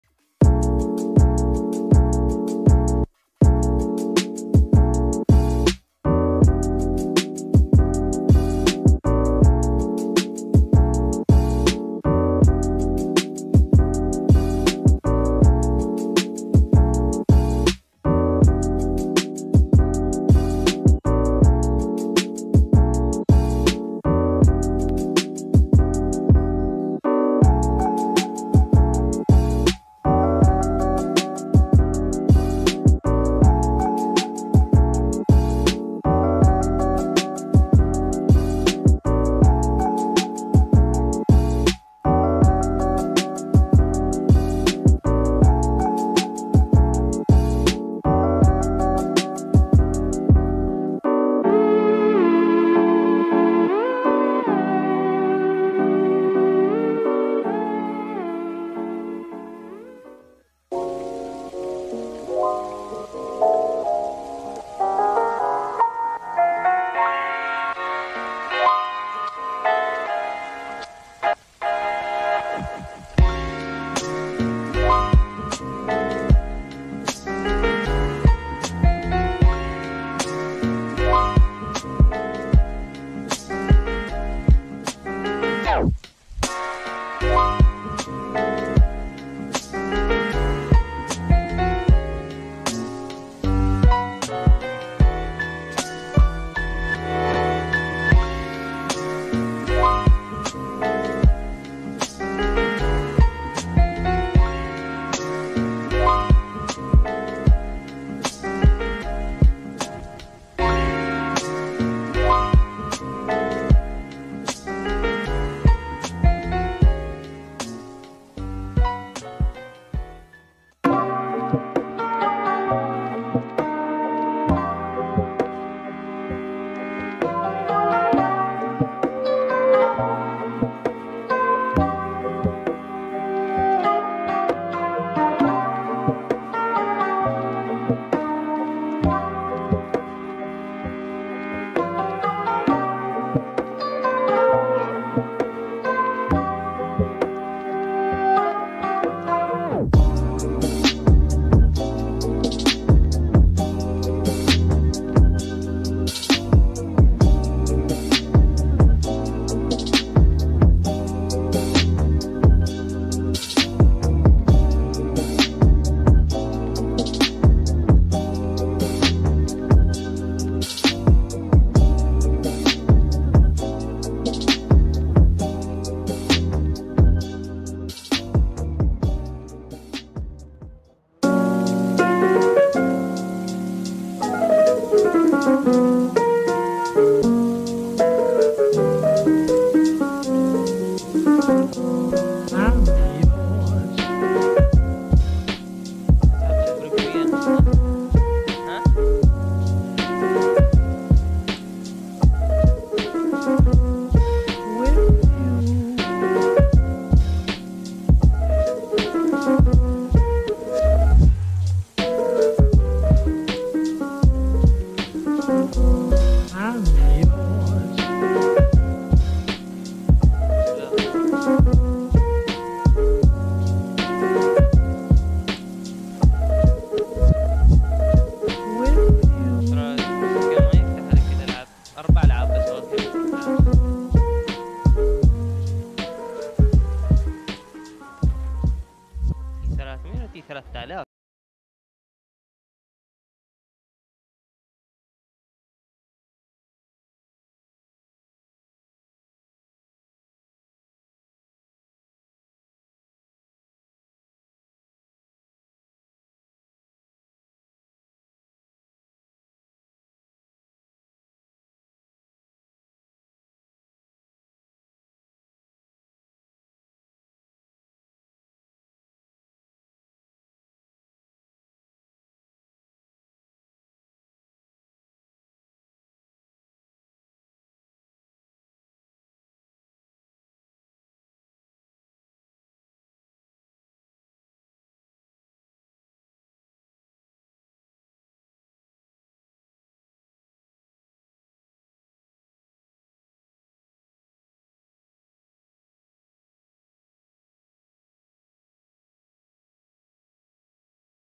NEPTUN GLITCH - After Effects AMV Tutorial
zneptun one frame tutorial , enjoy this after effects amv tutorial

شرح انتقالات زنبتن افتر افكت

0:00 intro
#1 00:08
02:35 Shake

Thumbnail anime : Demon Slayer, rengoku

, v1sper cc
neptun ,
zneptun
bars after effect
SEAMLESS